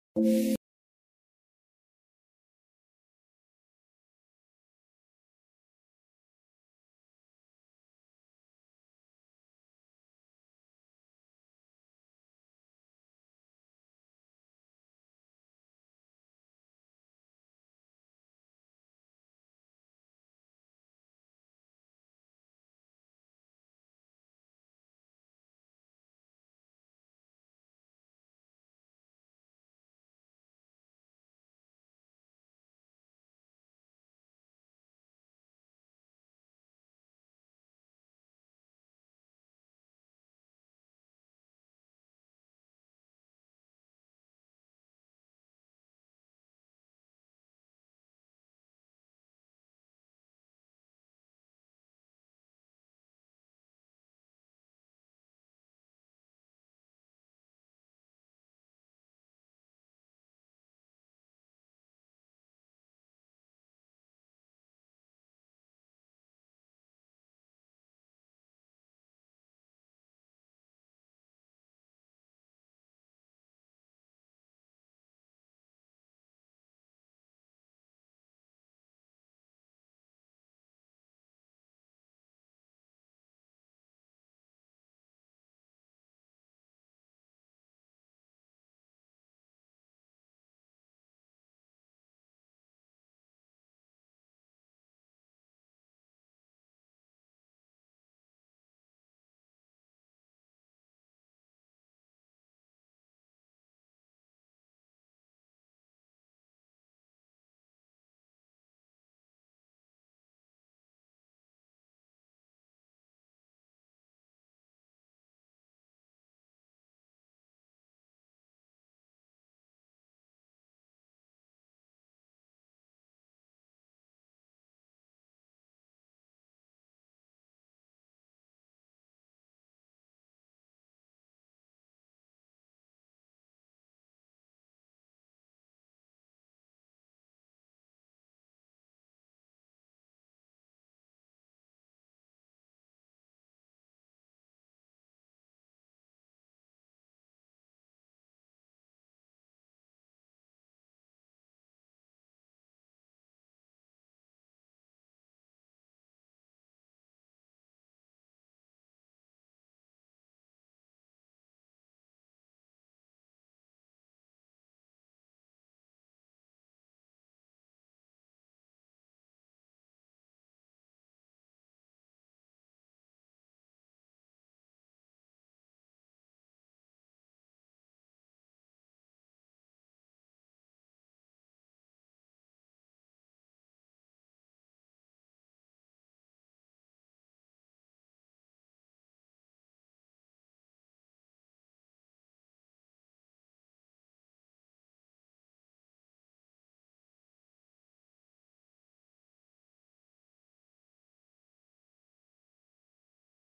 How to make Mini Portable Notebook at home _ DIY mini Notebook _ School hack
Very easy way to make paper notebook.